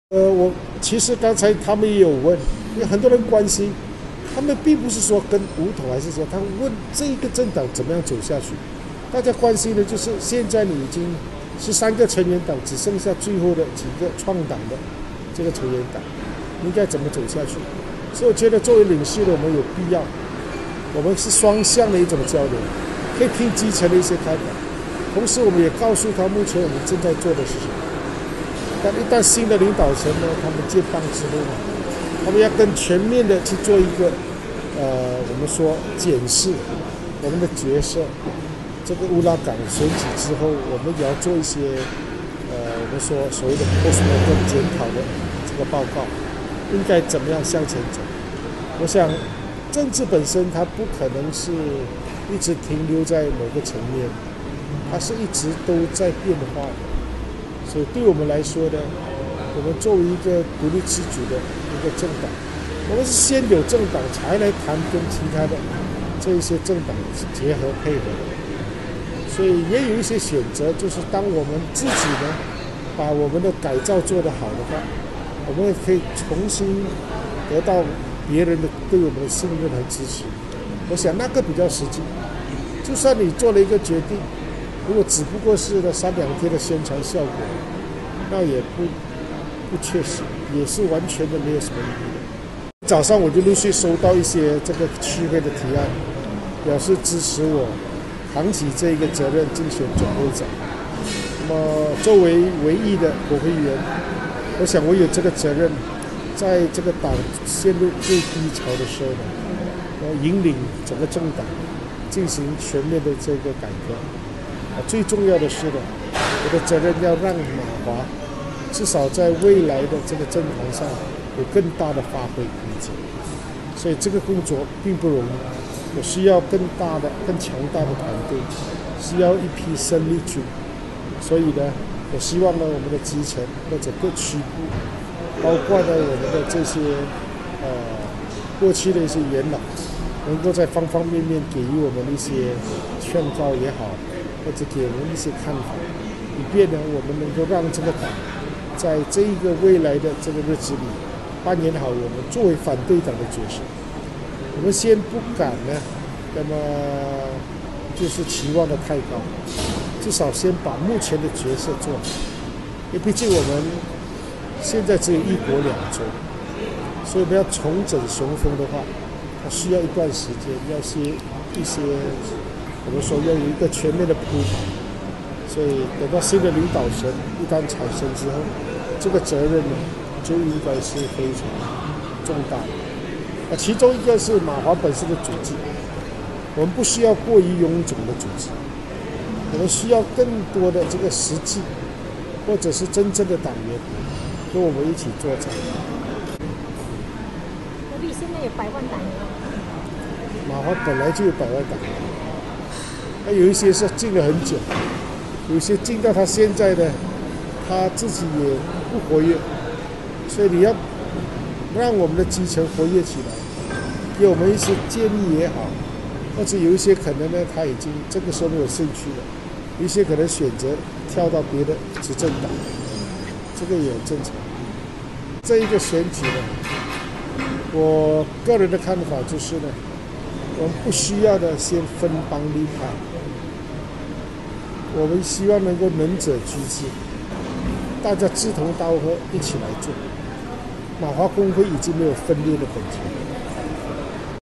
不談搭擋避免分派 魏家祥攻總會長
馬華署理總會長拿督斯里魏家祥表明會競選總會長職，作為唯一的馬華國會議員，有責任在黨處於低潮期，引領黨全面改革，讓馬華未來擁有更大發揮空間。（本報張來星攝）